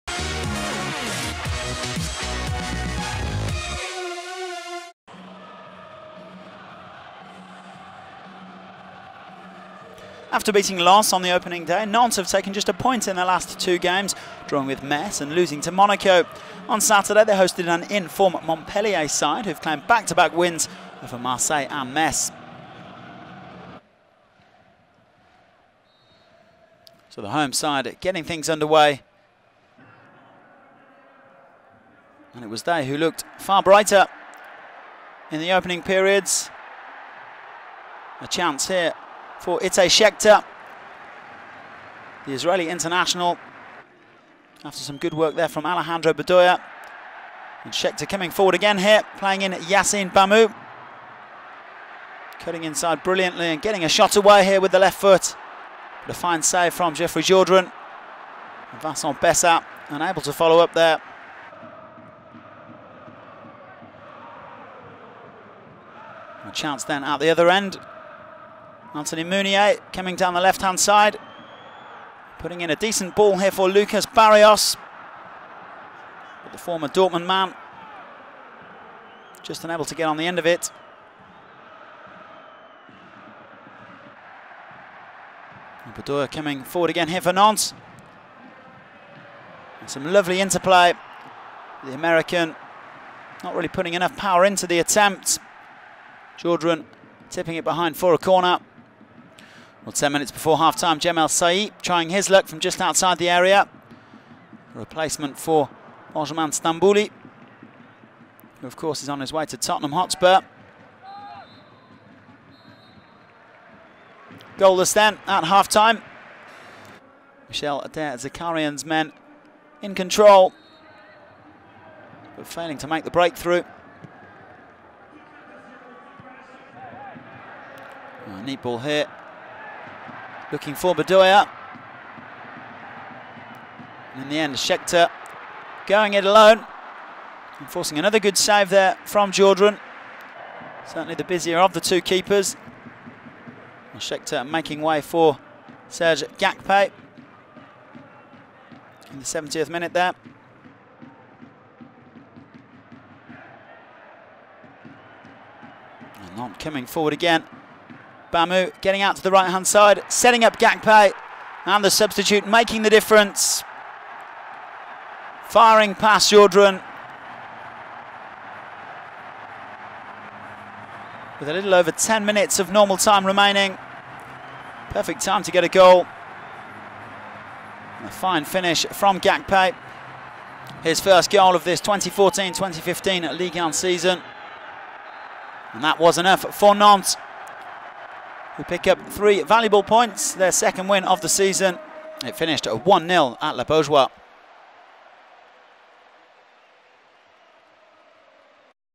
FC Nantes - Montpellier Hérault SC (1-0) - Highlights - (FCN - MHSC) / 2014-15
FC Nantes vs Montpellier Hérault SC (1 - 0) highlights. The best actions and goals of FC Nantes vs Montpellier Hérault SC in video.

Stade de la Beaujoire-Louis Fonteneau - Sat, 30 Aug 2014

Goals : Serge GAKPE (78' - FC Nantes)

FC Nantes Team line-up : 2 - Kian HANSEN / 3 - Papy Mison DJILOBODJI / 4 - Oswaldo VIZCARRONDO / 5 - Olivier VEIGNEAU (c) / 7 - Alejandro BEDOYA / 8 - Vincent BESSAT / 9 - Itay SHECHTER / 23 - Yacine BAMMOU / 24 - Chaker ALHADHUR / 25 - Jordan VERETOUT / 30 - Maxime DUPE